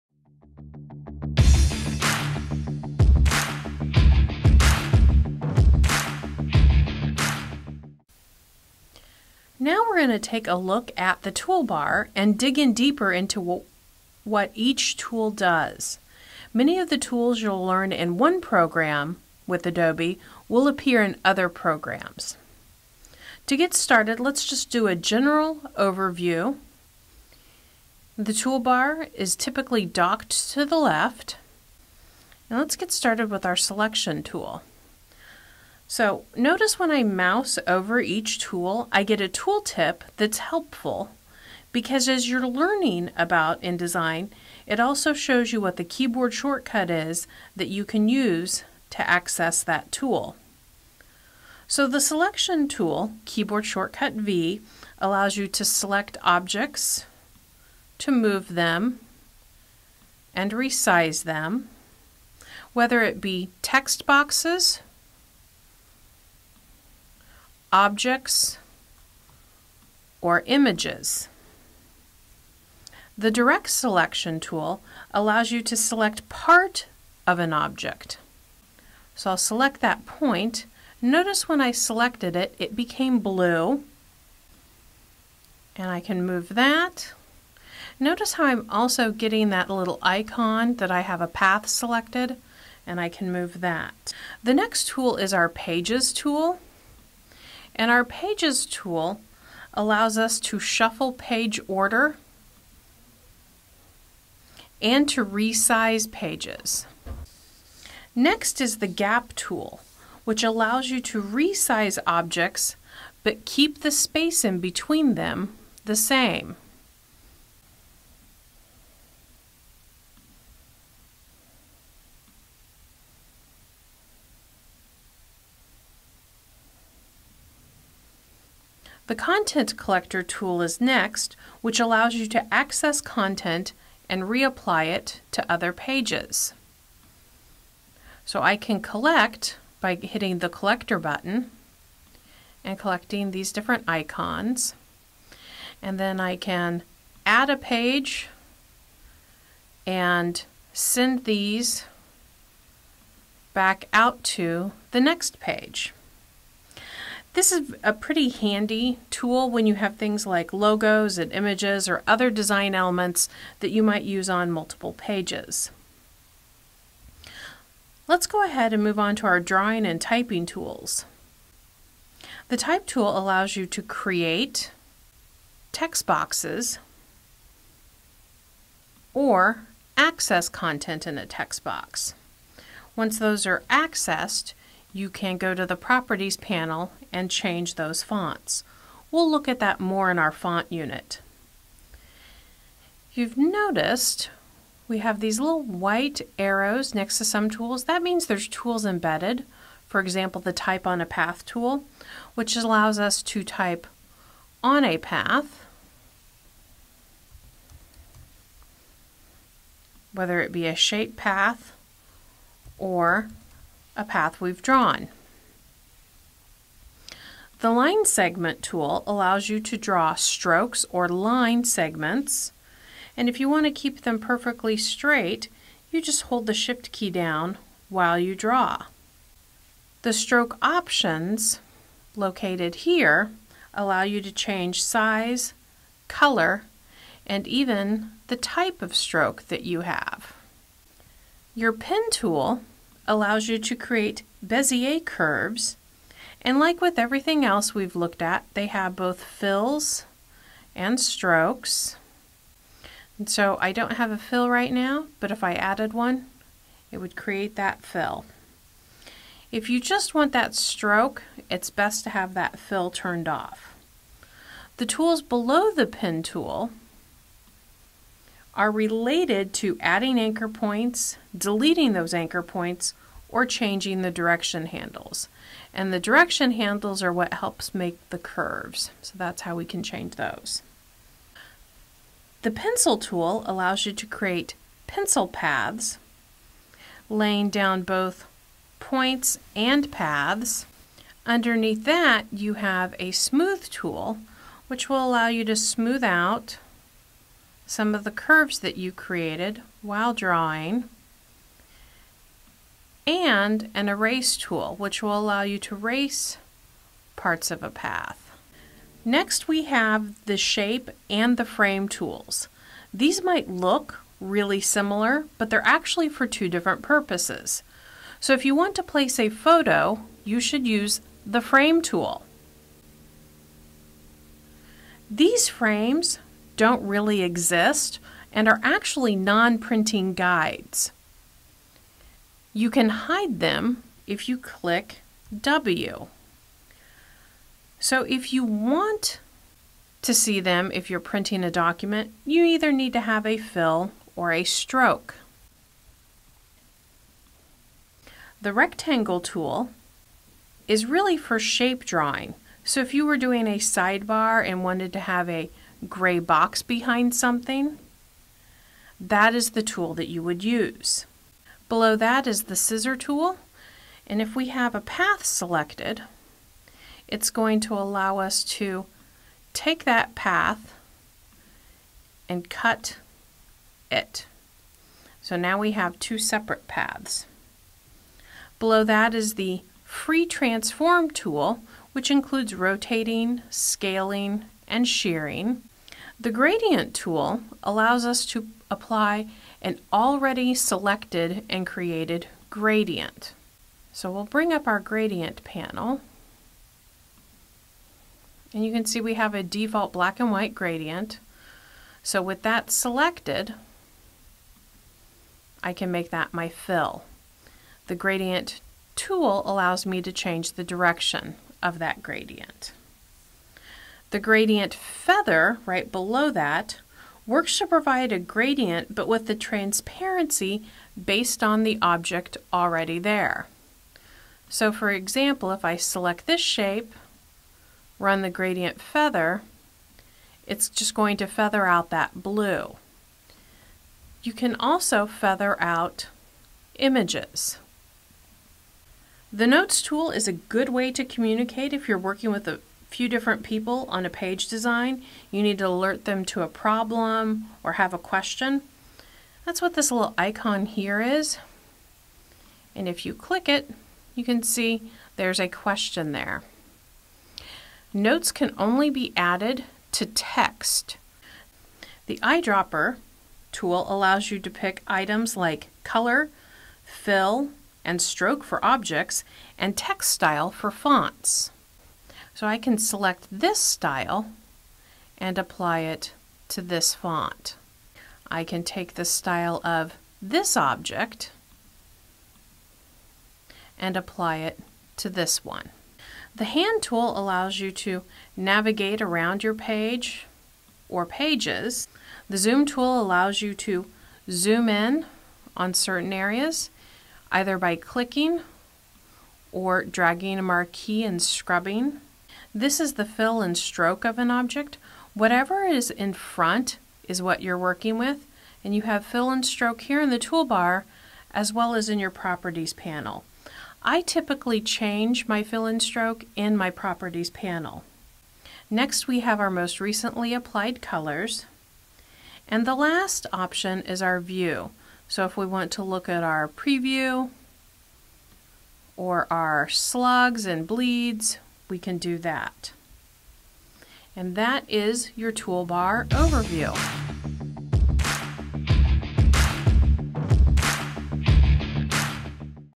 InDesign toolbar
Table of Contents:

00:41 - Learning Tools and Their Shortcuts
00:51 - Selection Tool
01:12 - Direct Selection Tool
01:35 - Pages Tool
01:48 - Gap Tool
02:08 - Content Collector Tool
02:51 - Type Tool
03:36 - Line Segment Tool
04:00 - Pen Tool
05:08 - Shape and Frame Tools
06:01 - Scissor Tool
06:19 - Free Transform tool
06:33 - Gradient Tool
06:53 - Gradient Feather
07:18 - Notes Tool
07:41 - Eyedropper Tool
08:05 - Hand Tool
08:11 - Zoom Tool
08:22 - Fill and Stroke
08:38 - Recently Applied Colors
08:42 - View Options